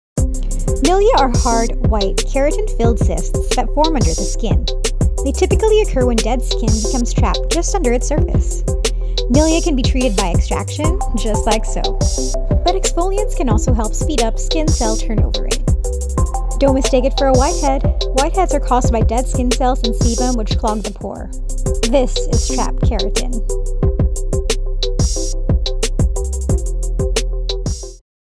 MILIA EXTRACTION ON THE FOREHEAD
Commonly mistaken as whiteheads, milia are benign, kertain-filled cysts that are more challenging to remove compared to whiteheads.

It's totally harmless but if you opt to remove it for cosmetic reasons, entrust your skin to the experts.

Our branches: Alabang Town Center, TriNoma, Greenbelt 5, Greenhills-Mandaluyong, Robinsons Place Manila, Vertis North, Ayala Malls the 30th and Marquee Mall.